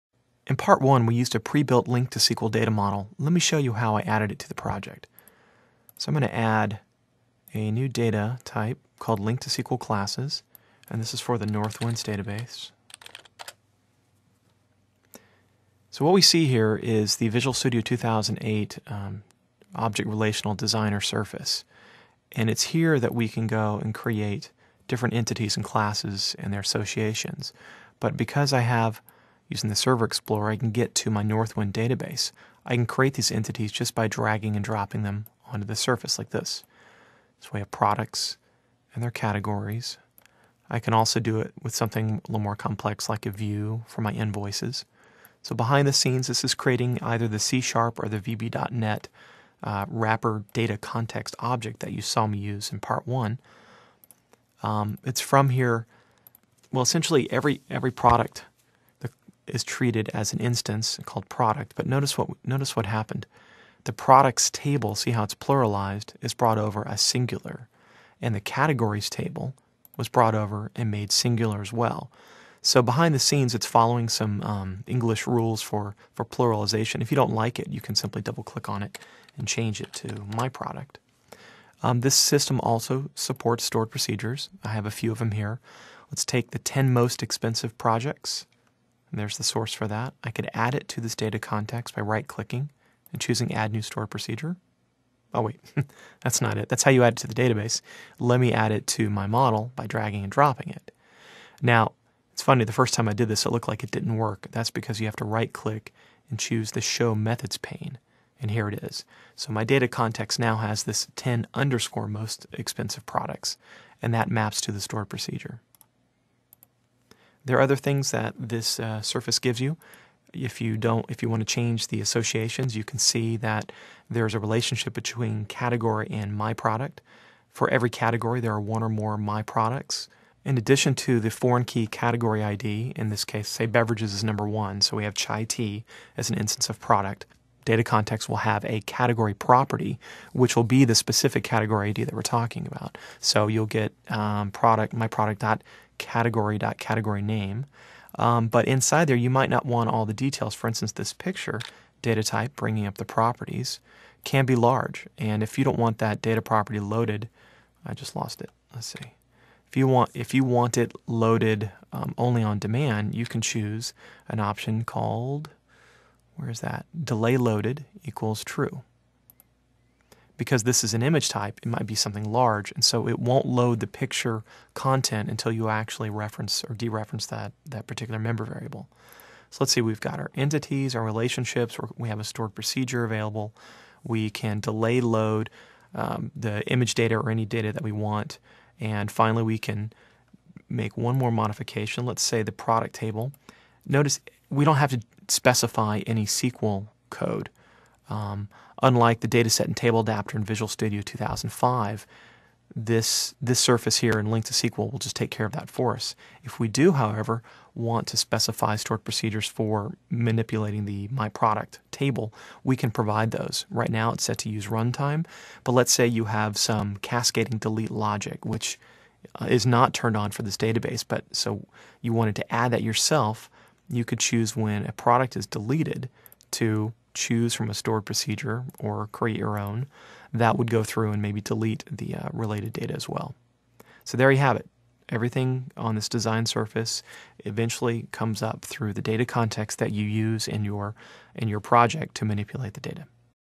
How to Use the LINQ to SQL Data Model
An overview of how to use LINQ to SQL's Data Model, courtesy of Microsoft's ASP.NET video series. To learn more about LINQ and Lambda expressions,